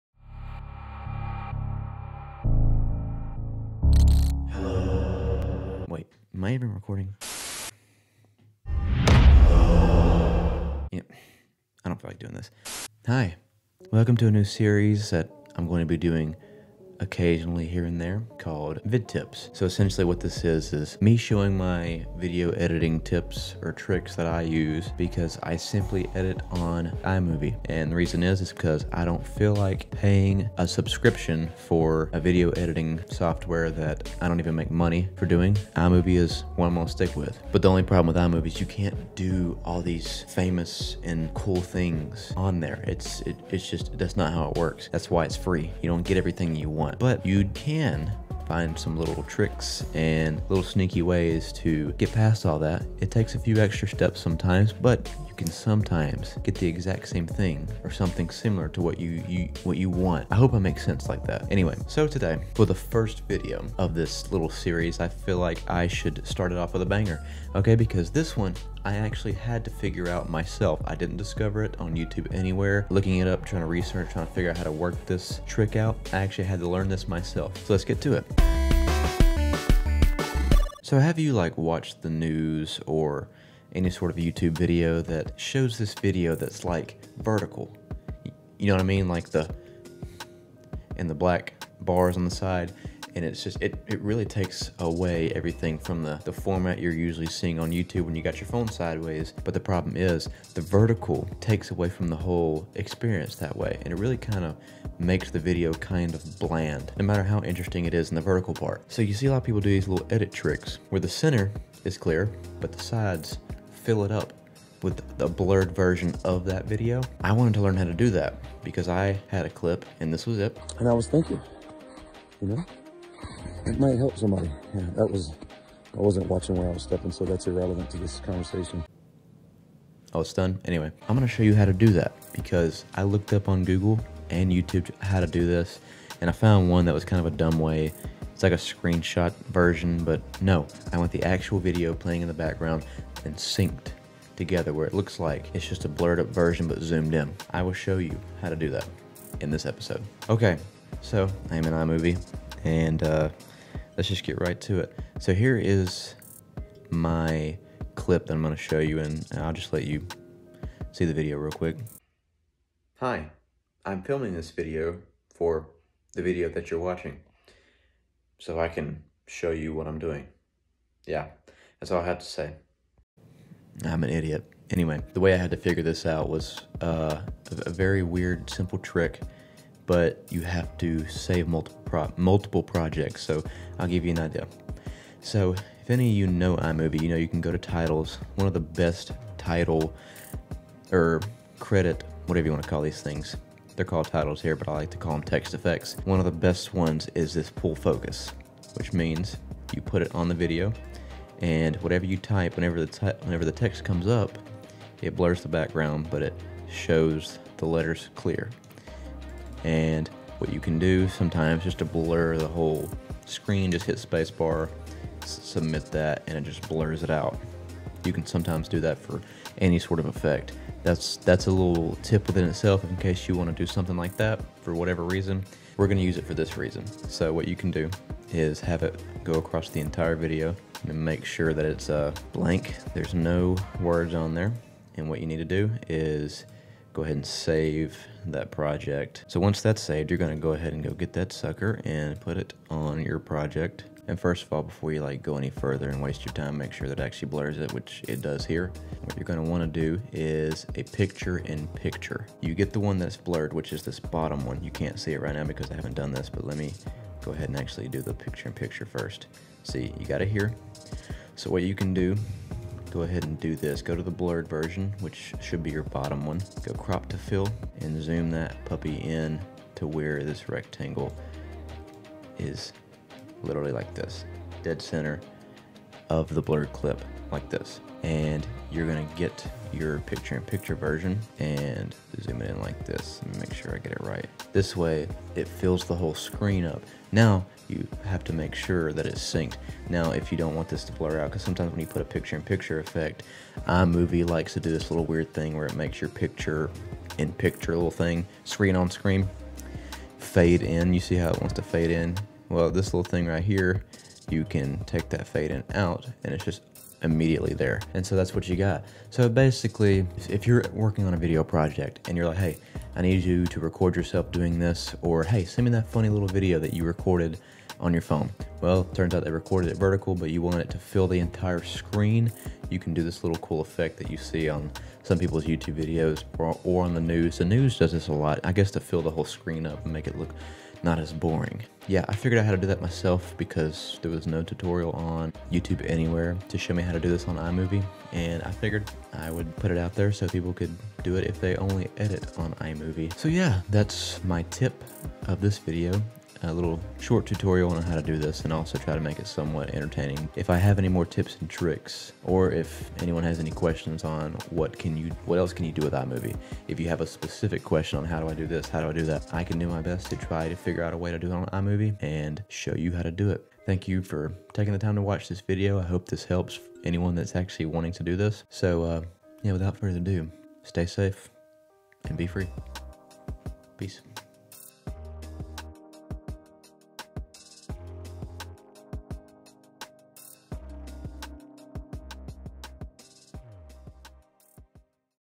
How to Fix Vertical Videos in iMovie
Please excuse how ‘down in the dumps’ I sound. I recorded this video at 1am.

Stay Safe
Be Free

Go watch my wife's first video on her channel!!